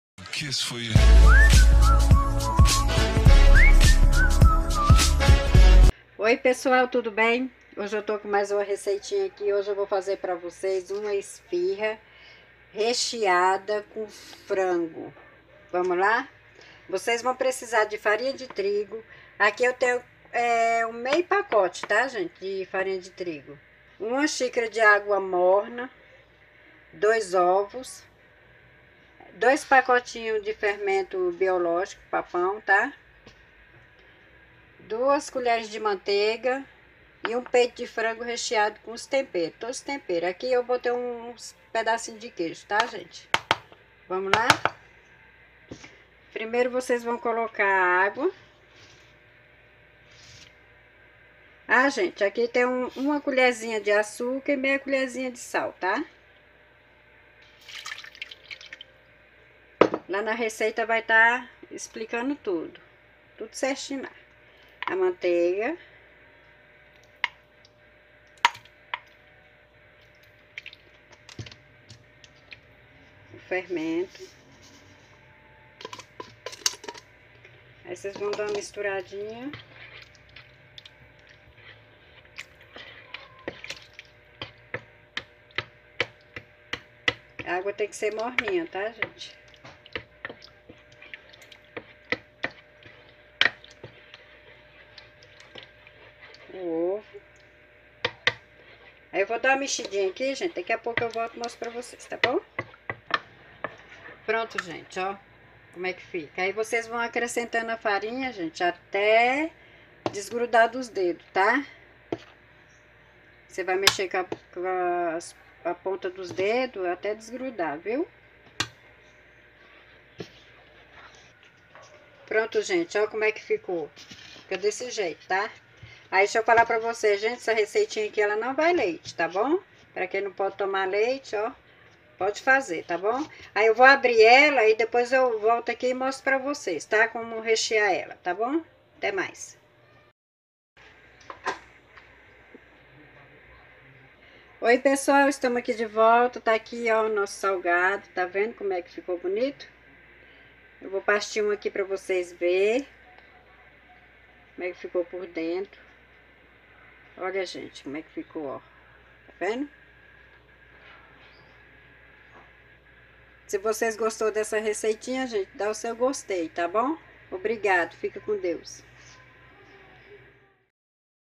COMO FAZER ESFIHA RECHEADA COM FRANGO ??
INGREDIENTES:
2 TABLETES DE FERMENTO.
1 COPO DE LEITE OU ÁGUA NORMA.
2 OVOS.
2 COLHERES (SOPA) DE MARGARINA.
1 COLHER (SOPA) DE AÇÚCAR, SAL E FARINHA DE TRIGO ( ATÉ AO PONTO DE ABRIR A MASSA)

                                   MODO DE FAZER:
DISSOLVA O FERMENTO NO LEITE OU ÁGUA MORNA, COLOQUE A MANTEIGA, OS OVOS, O SAL E A FARINHA DE TRIGO ATÉ QUE A MASSA SOLTE DOS DEDOS. COLOQUE NUMA VASILHA FUNDA E DEIXE CRESCER. FAÇA UM RECHEIO COM FRANGO COM TOMATES PICADINHOS EM PELE E SEMENTES, SAL, PIMENTA-DO-REINO E LIMÃO. ABRA A MASSA COM O ROLO - MAIS FINO E CORTE EM QUADRADINHOS. RECHEIE NO CENTRO, FECHE AS PONTAS, APERTE BEM, PINCELE CADA UMA COM GEMA. DEIXE CRESCER POR MAIS ALGUNS MINUTOS E ASE EM FORNO MÉDIO.
Quer me mandar algo ,algo mesmo que seja minimo , minha caixa postal abaixo:
caixa postal:7827.
cep:72305970.